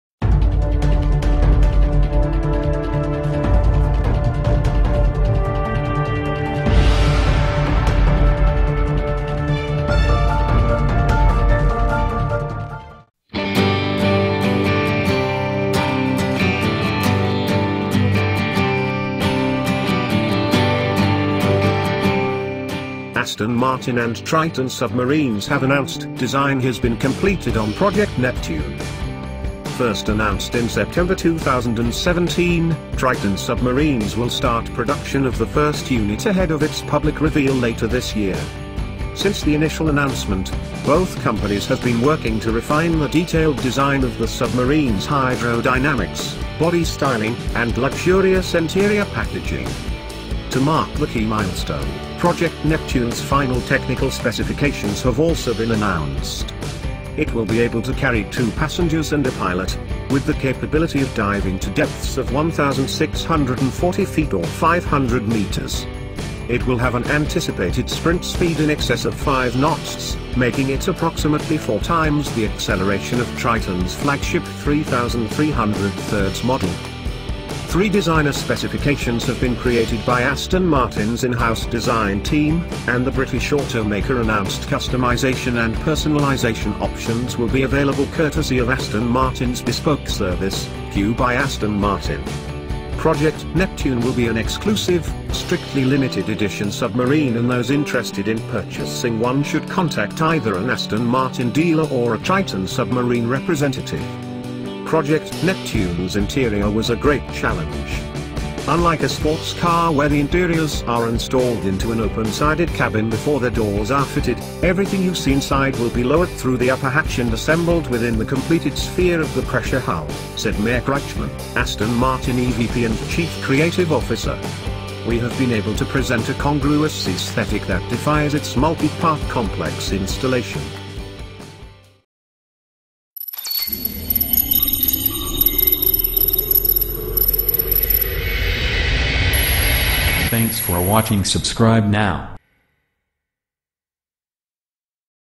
An Aston Martin Submarine is Going to Become Real
Aston Martin and Triton Submarines have announced design has been completed on Project Neptune.

First announced in September 2017, Triton Submarines will start production of the first unit ahead of its public reveal later this year. Since the initial announcement, both companies have been working to refine the detailed design of the submarine’s hydrodynamics, body styling, and luxurious interior packaging.

To mark the key milestone, Project Neptune’s final technical specifications have also been announced. It will be able to carry two passengers and a pilot, with the capability of diving to depths of 1,640 feet or 500 meters. It will have an anticipated sprint speed in excess of five knots, making it approximately four times the acceleration of Triton’s flagship 3300/3 model.

NewCarGallery MyChannel